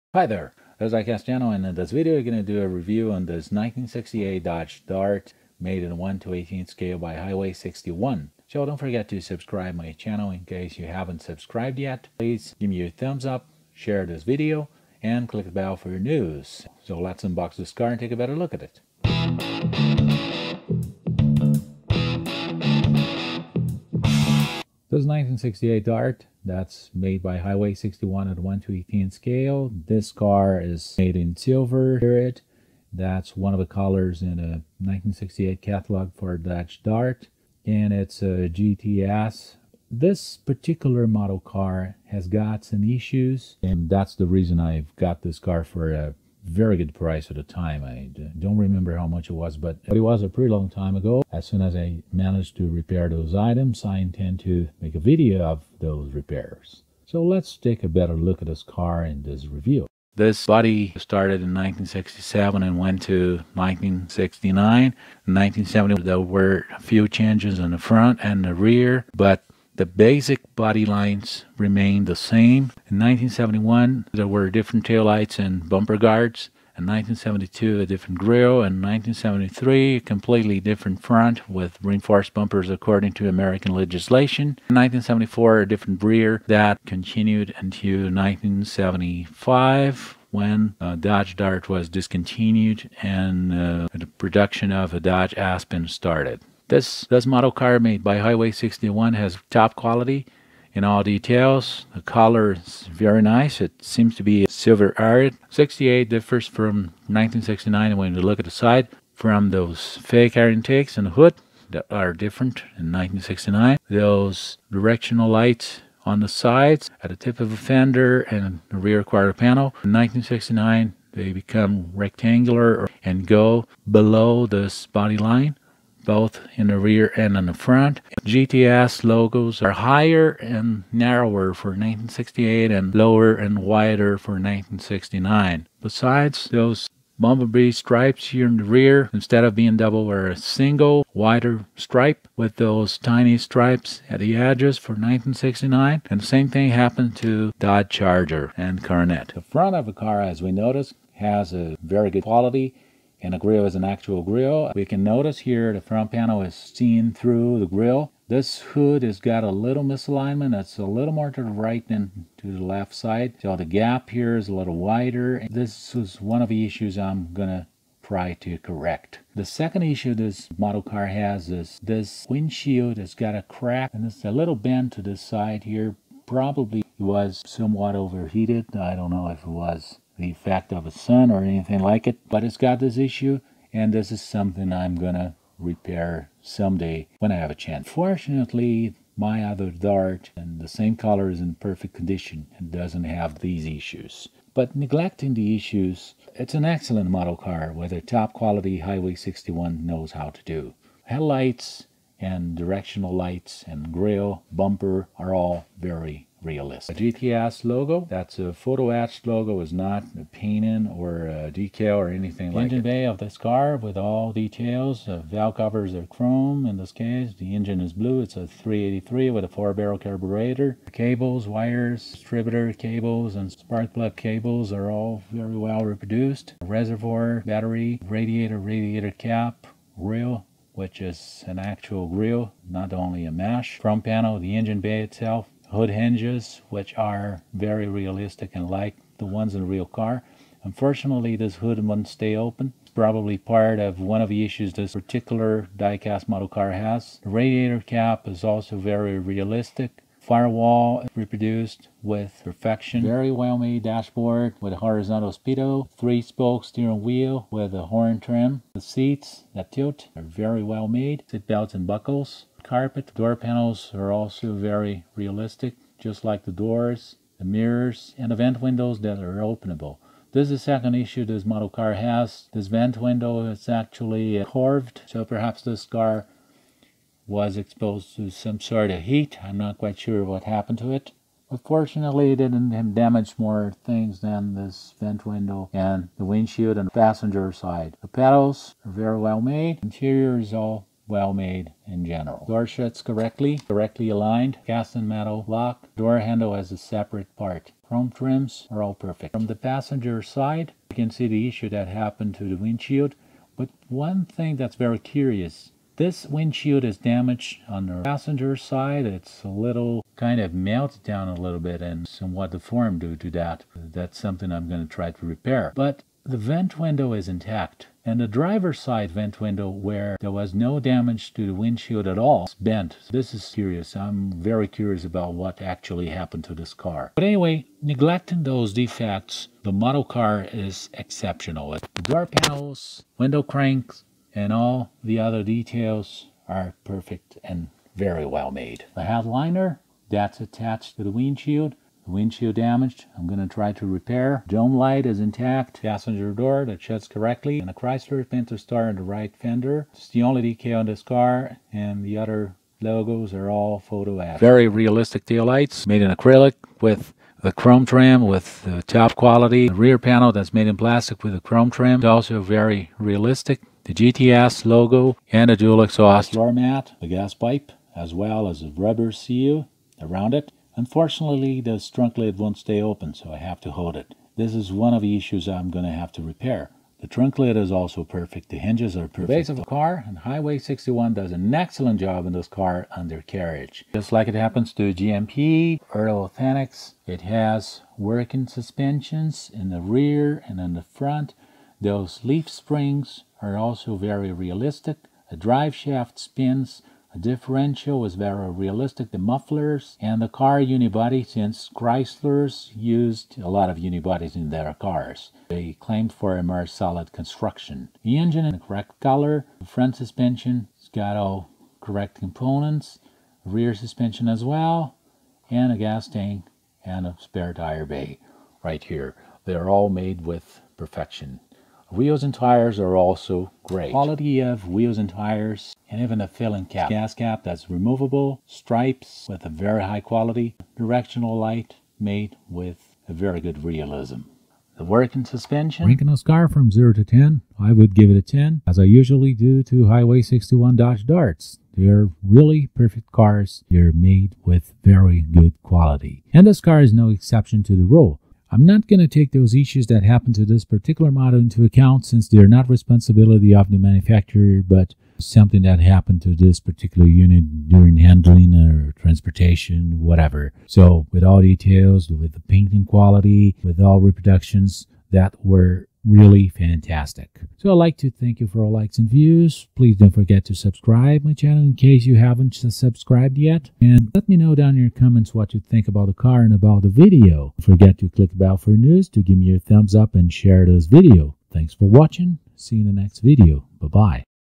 Review of a 1:18 Highway 61 1968 Dodge Dart GTS
In this review, I unbox and analyze a 1:18 Highway 61 1968 Dodge Dart GTS that, despite some issues, is a great diecast model car!